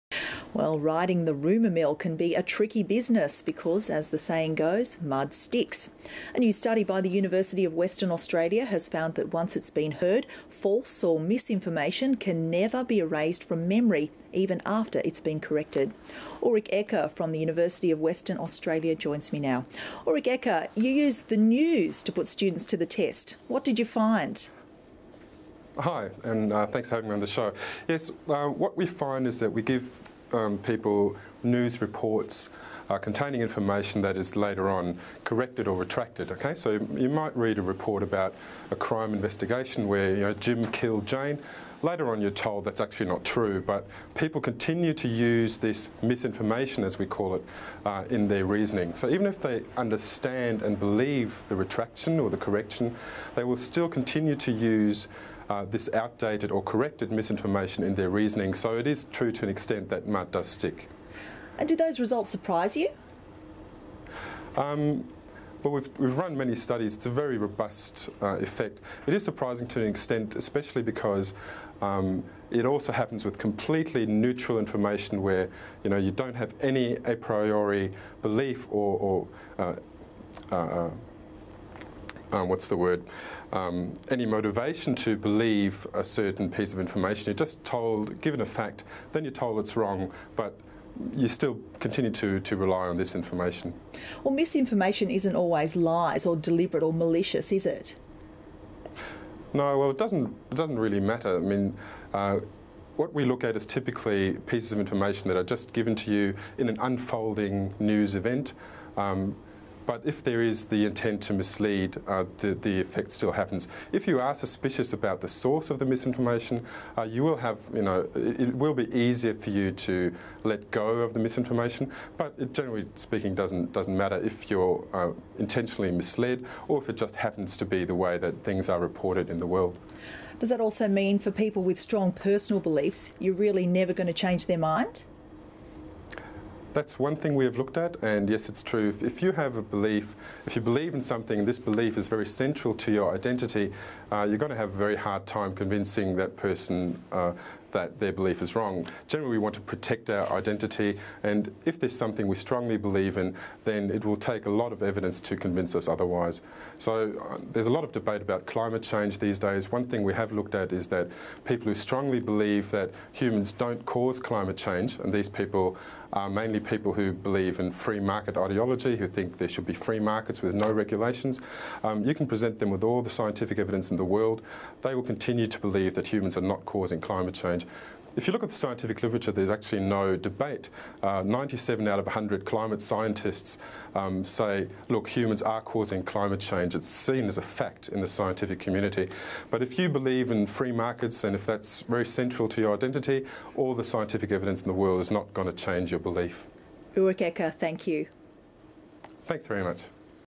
Misinformation impossible to erase, study finds
Ullrich Ecker, from the University of Western Australia, discusses a study that found false information cannot be deleted from memory even if corrected.